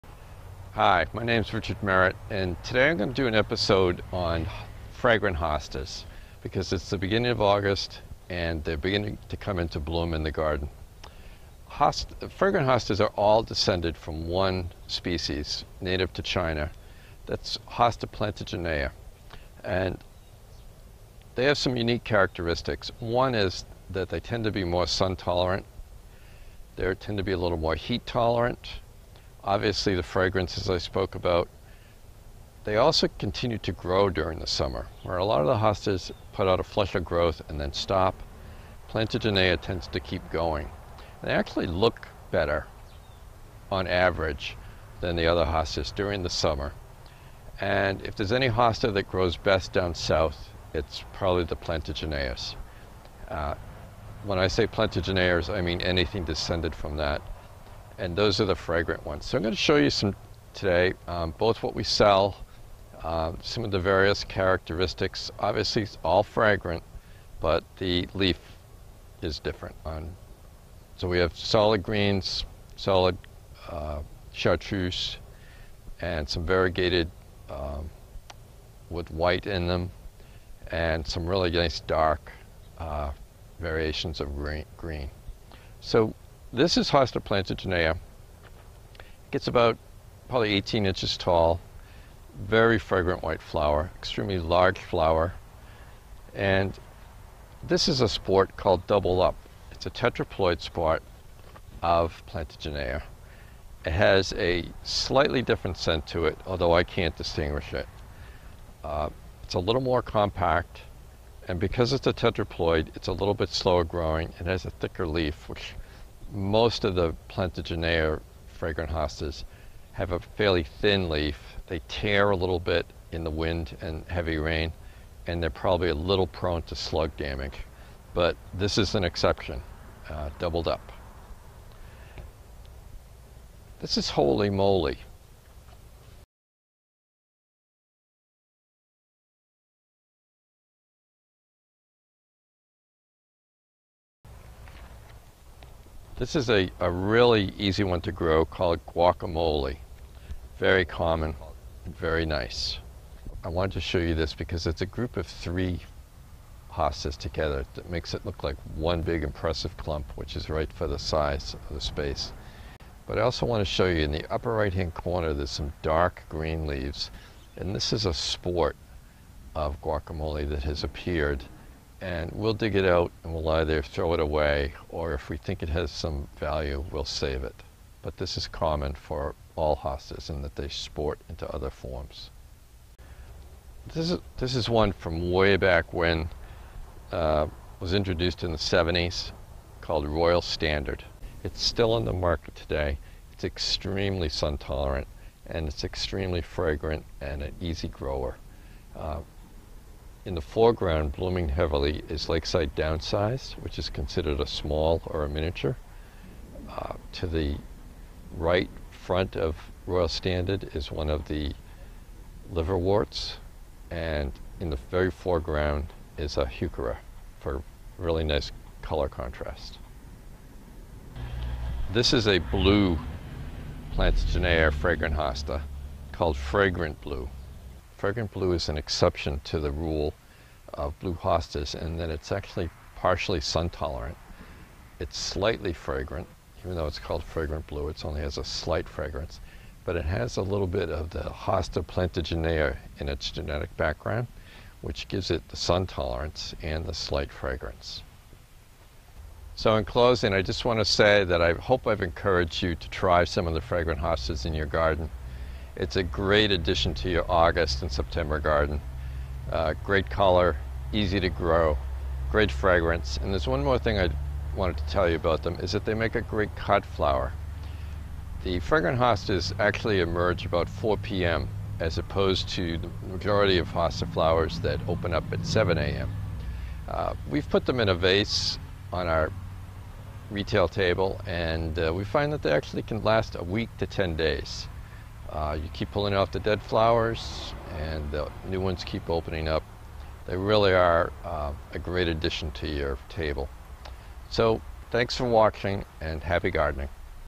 This Week In Our Hosta Garden: Fragrant Hostas
nhhostas.com - Fragrant hostas are summer's sweet reward to gardeners.  See our collection!  For those of you in the southern states that struggle with your hostas in the summer heat, the fragrant hostas perform better than other types in the heat. For the northern states, the fragrant hostas tend to perform better in the sun than other types.